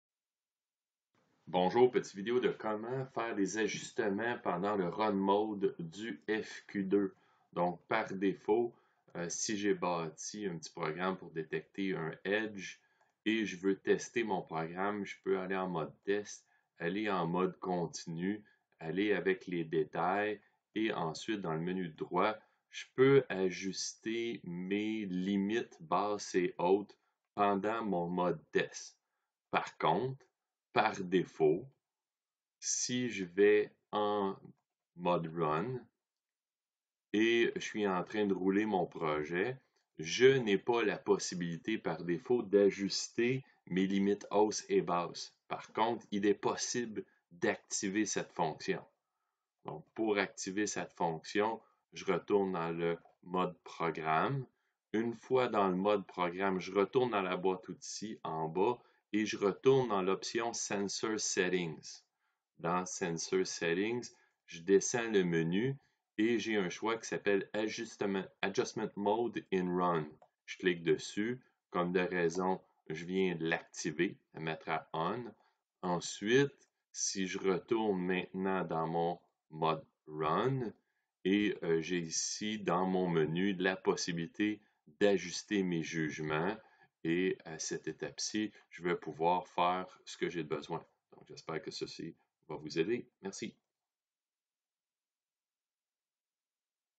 OMRON FQ2, tutoriel pour ajuster les niveaux d'acceptation en mode "RUN"
Par défaut le FQ2 ne permet pas aux opérateurs de faire des ajustements sur les niveaux d'acceptation des inspections.  Il est possible d'activer cette possibilité et ce vidéo vous explique comment